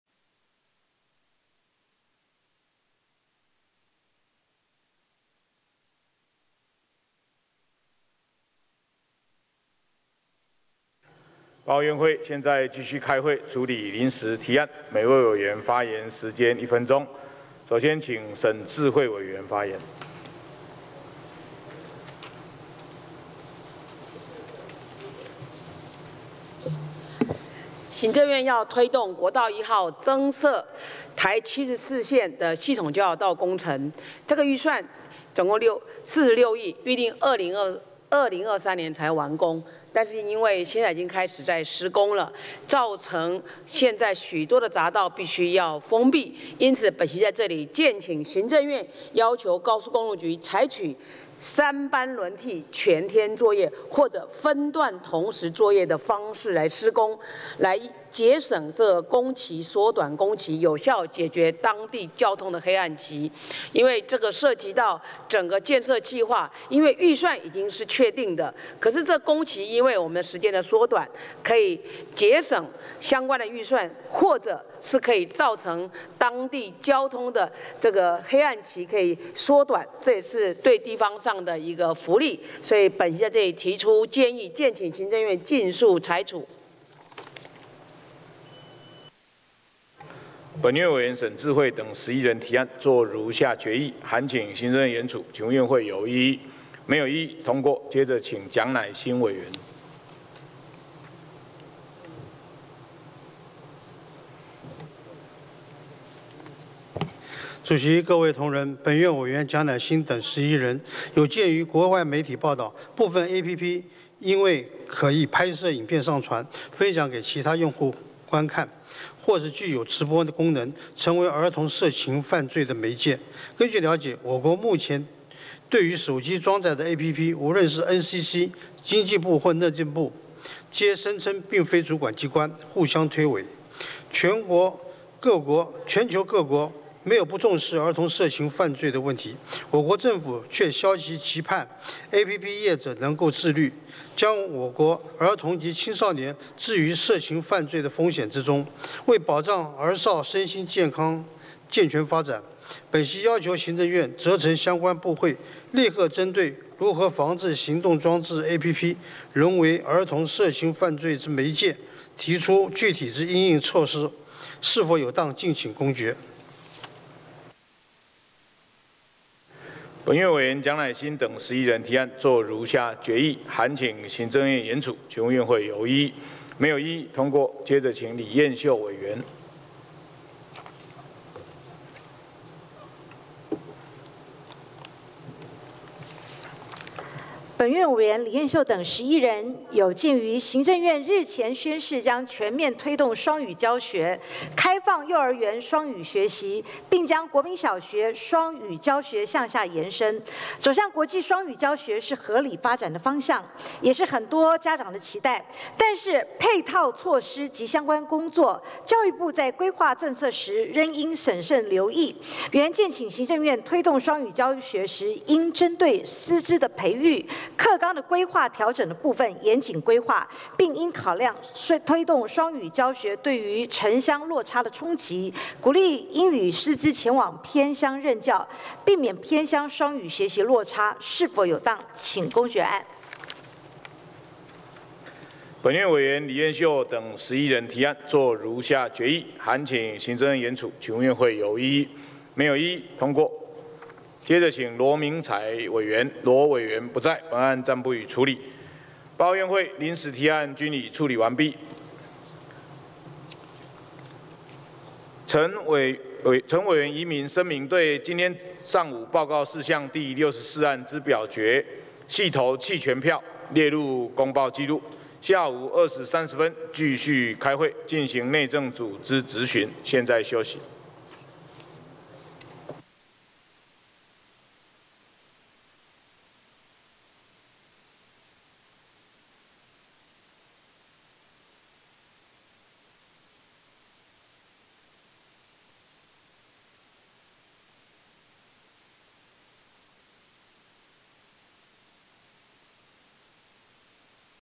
2019 03 05 院會 臨時提案 沈智慧 蔣乃辛 李彥秀 【立法院演哪齣？】
台一接74號 ＡＰＰ變兒童色情犯罪媒介 幼兒雙語教學
04:15 陳宜民聲明早上同婚專法逕付二讀 投棄權票